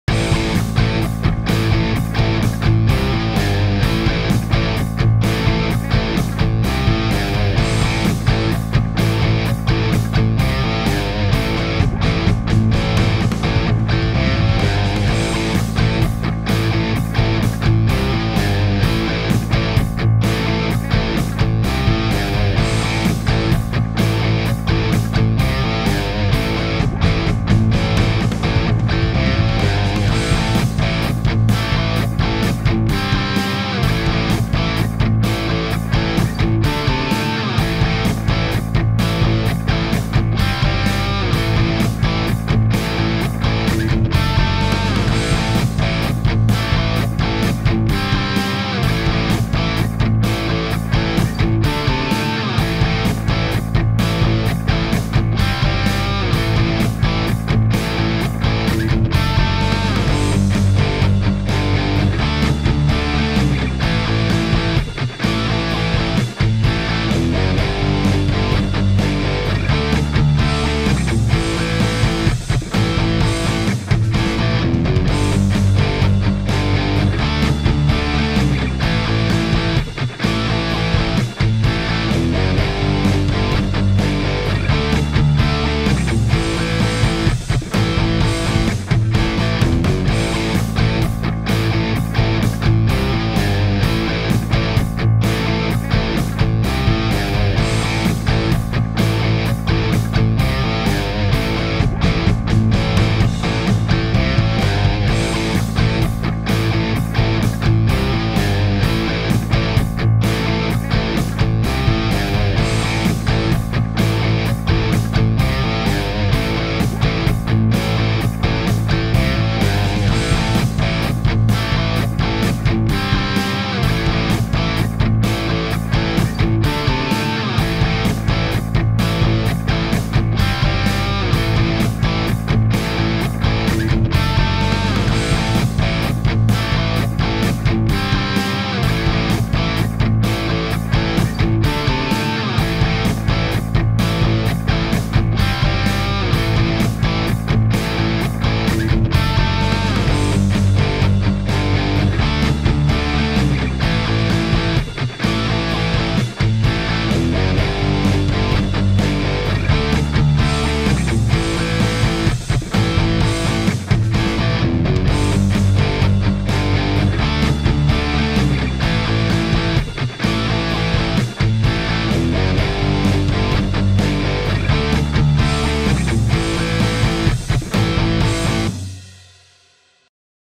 Heavy metal jam track in a minor
This heavy metal jam track is written in the key of a minor. Typically i would suggest to start soloing with a minor pentatonic, although you could also use a natural minor or a harmonic minor.